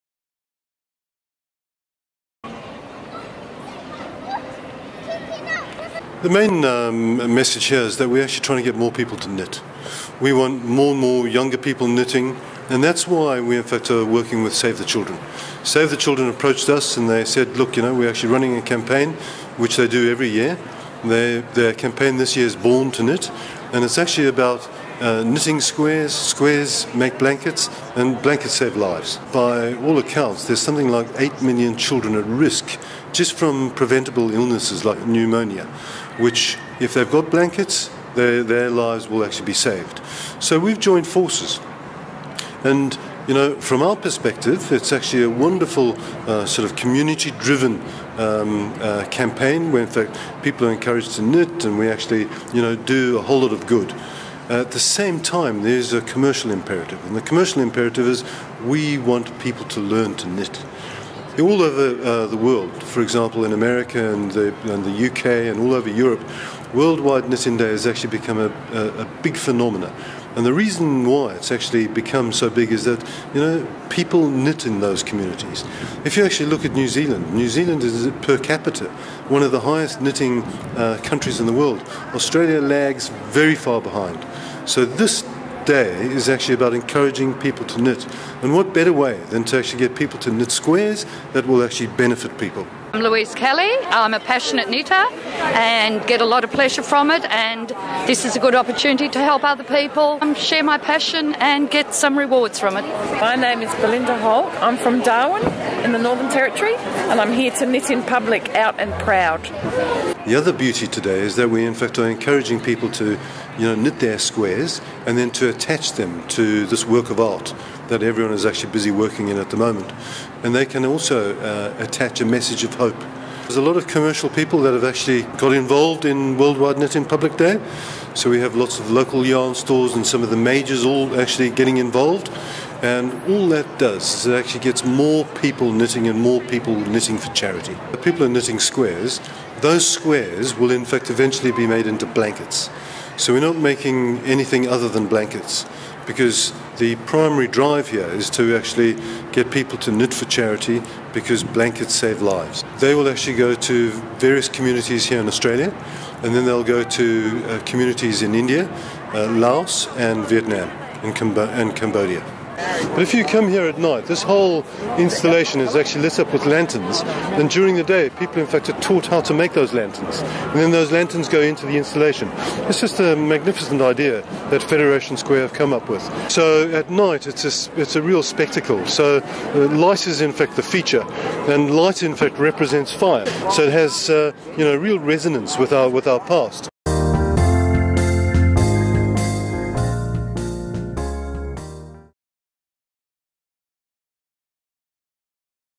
World Wide Knit In Public Day.mp4
June 2011 marks World Wide Knit in Public Day. That's right a day where we all hold our knitting needles high and carry our ball of wool with pride. A day to band together and click away. As the official tag line of the day says 'Better living through stitching together'.